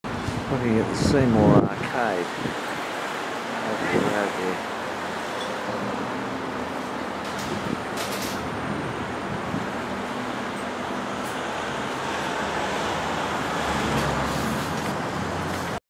MOV018 Seymour Arcade Traralgon Vic AUS 11-10-2013 (RGC198)
View of the Seymour Arcade in Seymour Street, Traralgon Vic Australia on October 11, 2013.